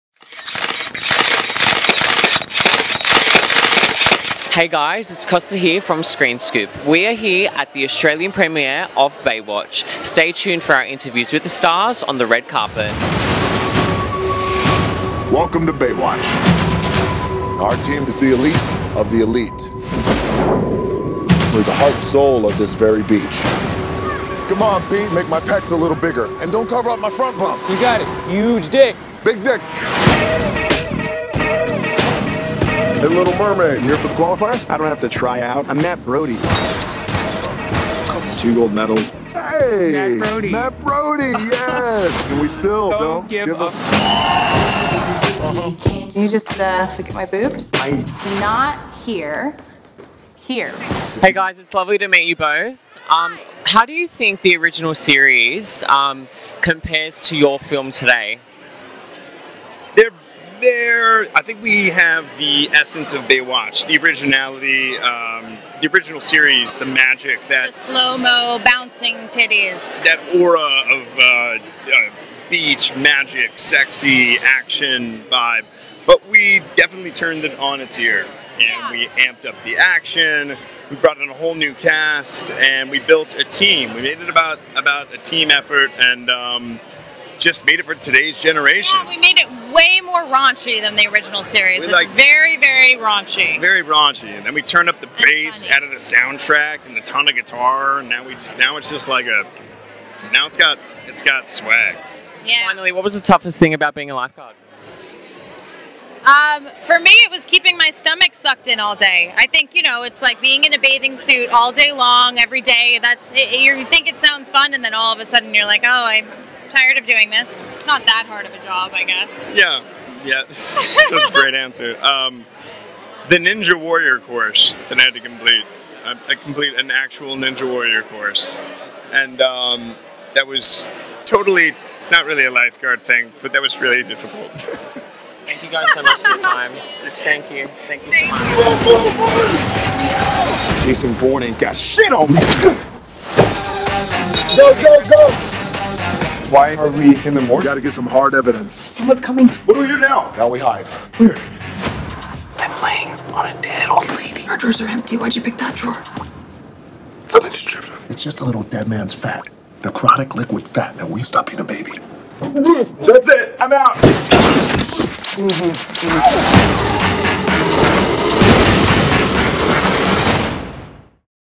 Baywatch Australian Premiere Interviews | Zac Efron & Alexandra Daddario
Check out our coverage from the Baywatch Australian Premiere with Zac Efron and Alexandra Daddario which was held at Sydney's Hoyts Entertainment Quarter!

Baywatch hits Australian cinemas on June 1, 2017!